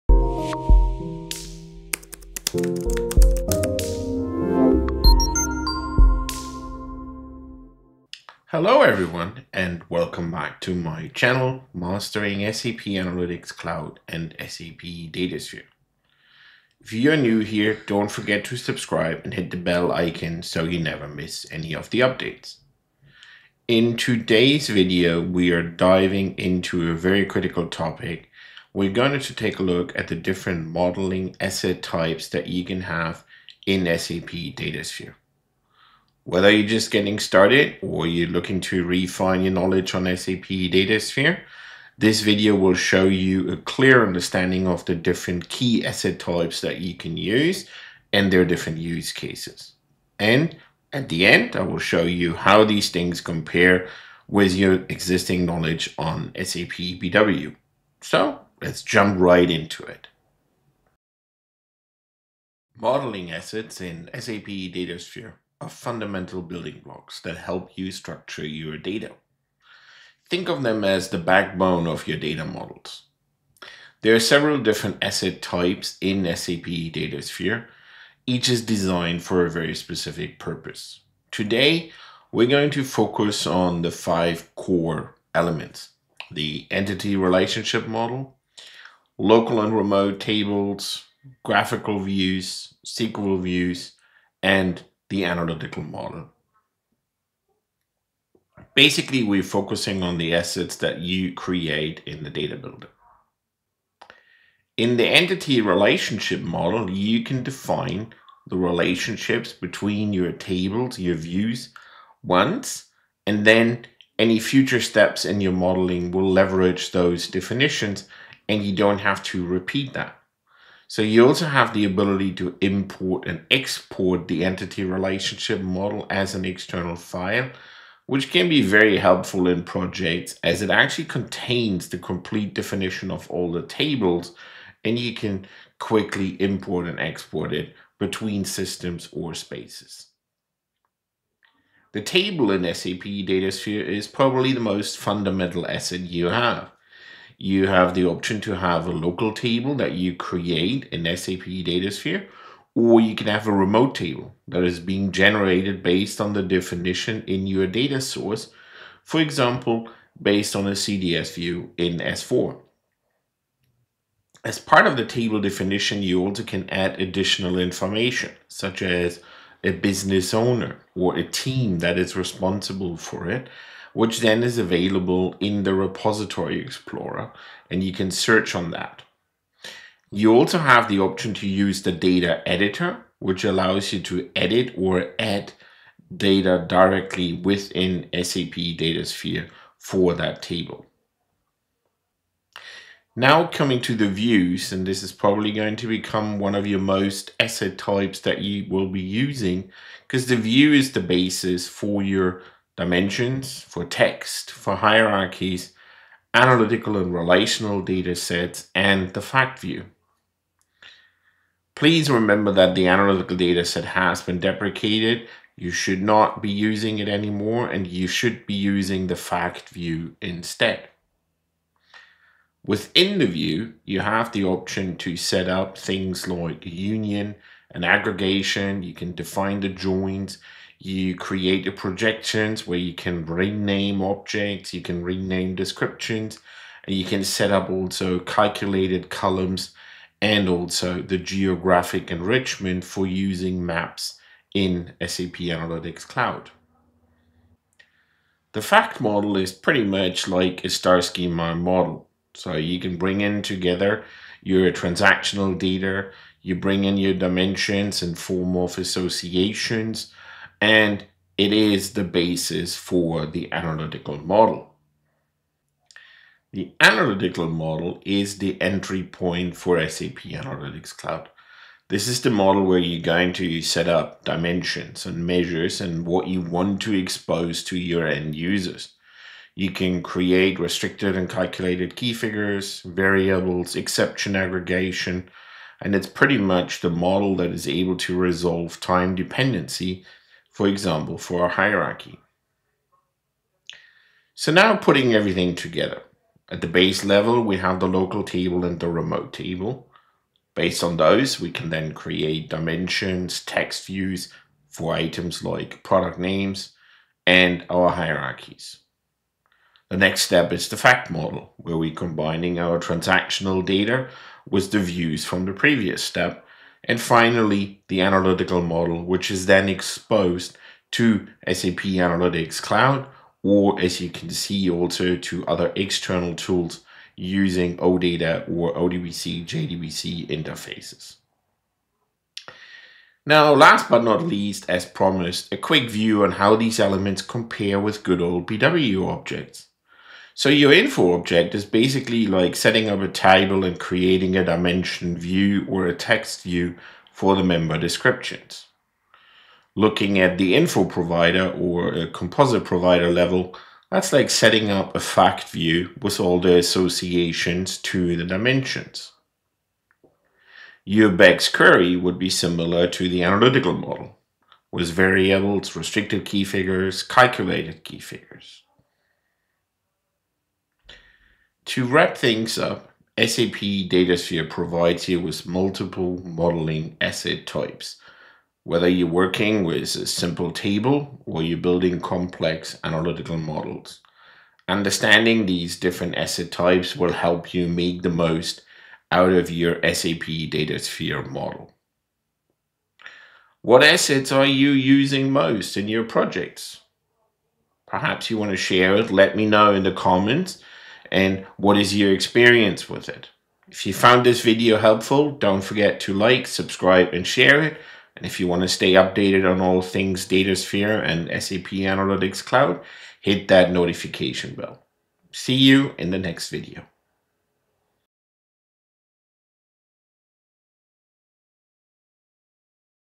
SAP Datasphere Modeling Asset Types Explained: Tables, Views, Analytical Models & More!
🚀 Master SAP Datasphere Modeling Asset Types! Whether you're new to SAP Datasphere or looking to optimize your data models, this video covers the essential modeling asset types—including Tables, Views, Analytical Models, Business Entities, and Graph Workspaces—and how they fit into your data modeling strategy.
💡 In this in-depth guide, you'll learn:
✔ What are modeling assets in SAP Datasphere?
✔ How to use Tables, Views, and Analytical Models effectively
✔ The role of Business Entities for governance and semantics
✔ When to use Graph Workspaces for relationship-based data
✔ Best practices for data transformation and analytics in SAP Datasphere
📌 Chapters:

0:00 - Introduction
1:02 - What are Modeling Assets in SAP Datasphere?
1:44 - Entity Relationship Model
2:22 - Tables: The Foundation of Data Modeling
3:12 - Views: Transforming Data for Analytics
4:07 - Analytical Models: Powering Business Insights
4:53 - Putting things together
5:32 - SAP BW vs SAP Datasphere modeling assets

💬 Which SAP Datasphere asset type do you use the most?